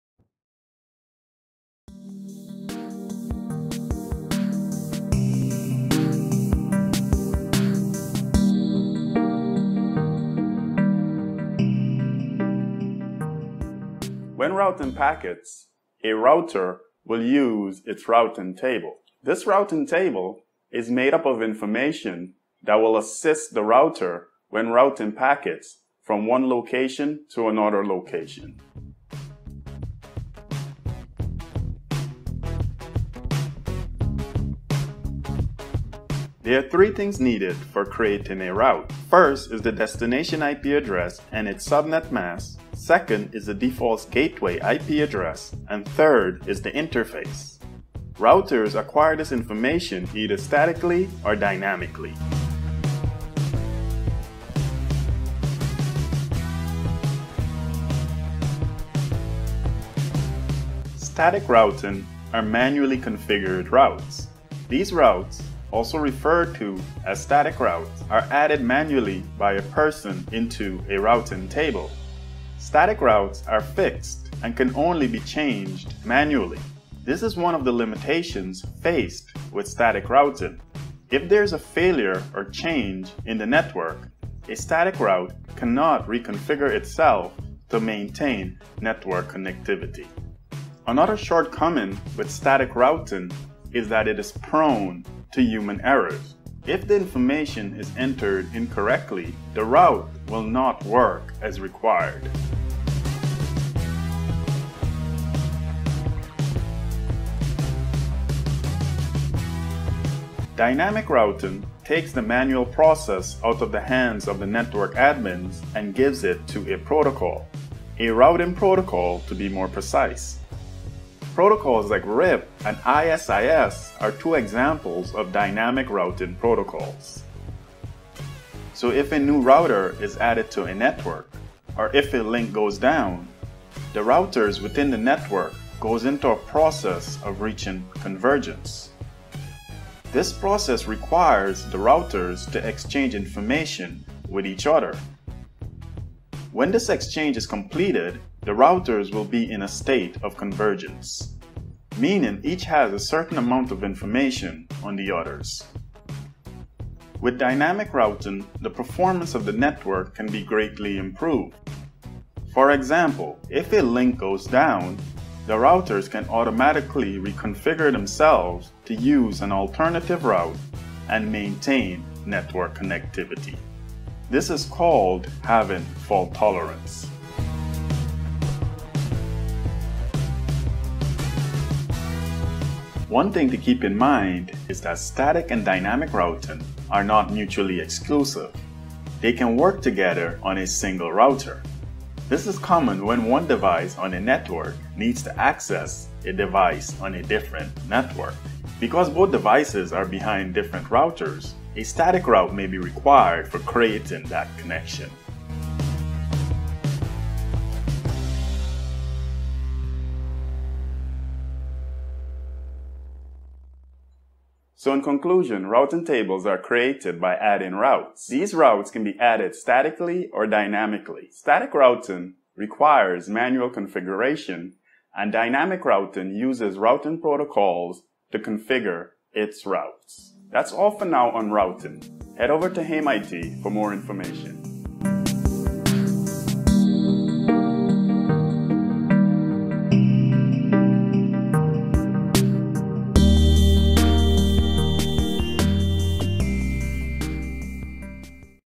Routing— Networking Paths
When routing packets, a router will used its routing table. This routing table is made up of routing information that will assist the router when routing packets from one location to another location. There are three things needed when routing packets; a destination Ip address and its subnet mask, the default gateway's Ip address, and the interface. Routers acquire this information either statically or dynamically.